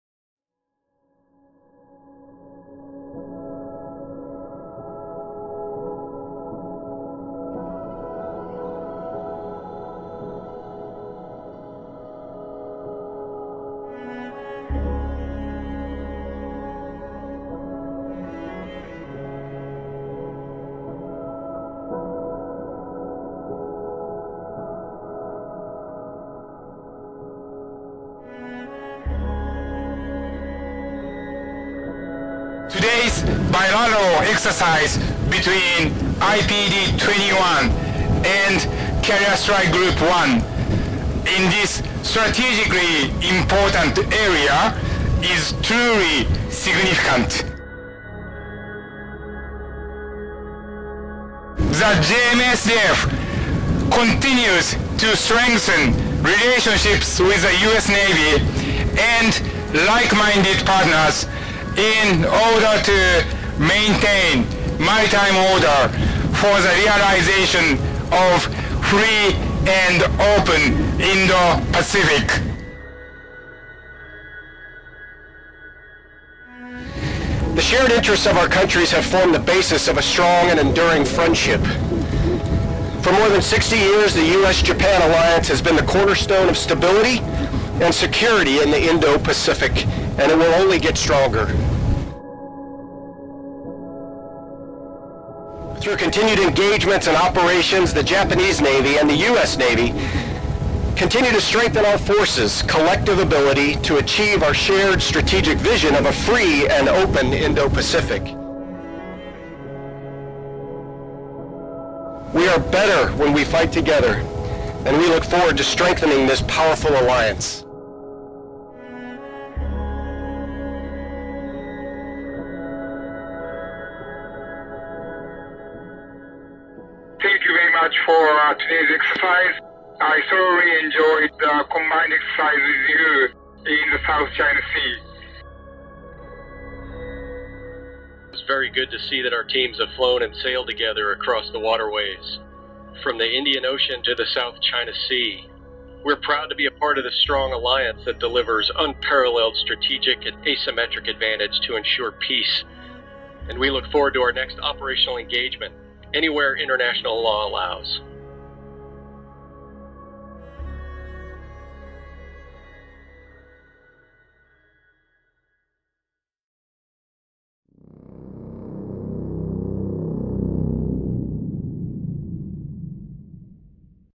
Commander JMSDF Escort Flotilla 3 & US Carrier Strike Group 1& Bilateral Ops in South China Sea
SOUTH CHINA SEA (Oct. 30, 2021) Rear Adm. Izuru Ikeuchi, commander, Escort Flotilla Three (CF3), and Rear Adm. Dan Martin, commander, Carrier Strike Group (CSG) 1, speak about recent operations between CF3 and CSG 1 from the flight deck of Japan Maritime Self-Defense Force (JMSDF) Izumo-class helicopter destroyer JS Kaga (DDH 184), Oct. 30, 2021. Elements of JMSDF and CSG 1 are conducting bilateral operations in the South China Sea. (U.S. Navy video by Mass Communication Specialist 2nd Class Haydn N. Smith)